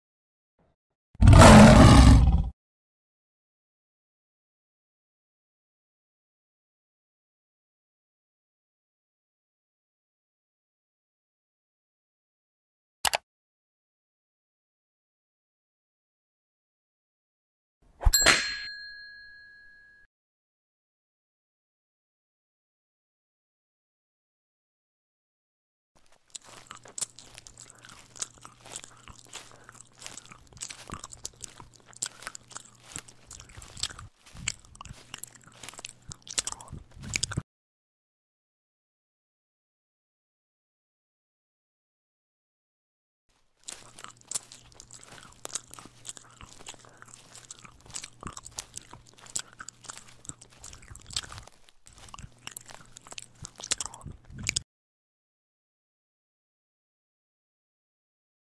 Horse Eating Grass
Just an animation of a horse eating grass :)